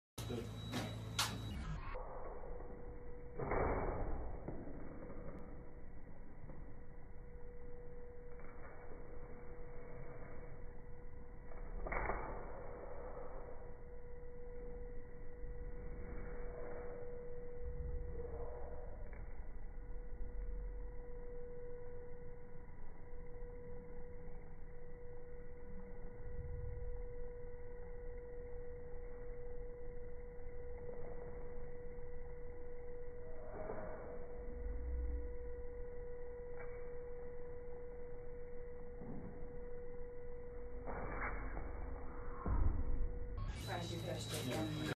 DIY Filmmaking Hack with a Cardboard Fan Blade on a Drill. BRILLIANT!
DIY Filmmaking Hack with a Cardboard fan blade on a drill with a cardi holding it and another cardi delicately pulling the trigger.